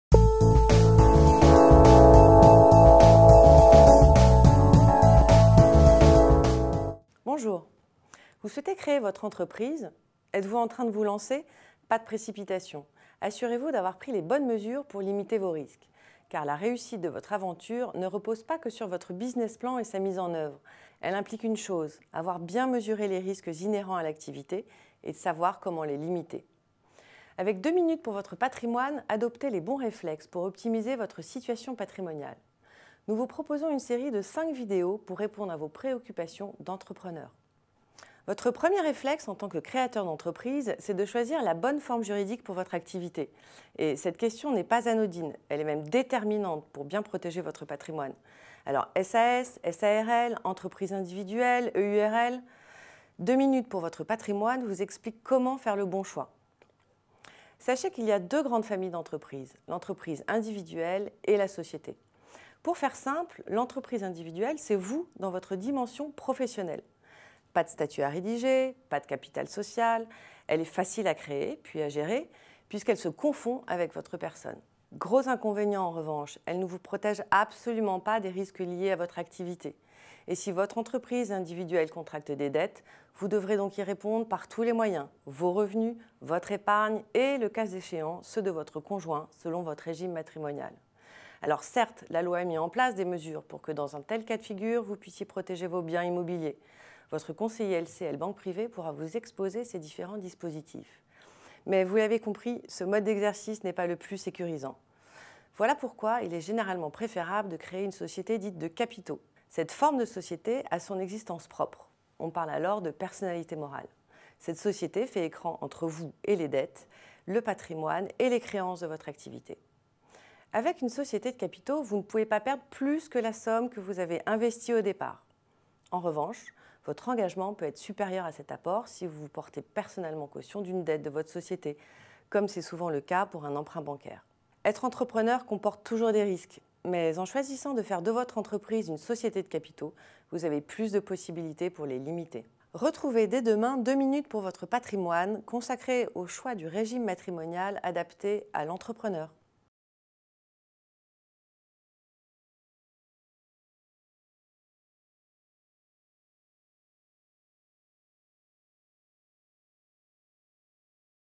Entrepreneurs Comment protéger votre patrimoine ? - LCL Banque Privée
Réflexe 1 : adopter la bonne forme juridique pour votre entreprise.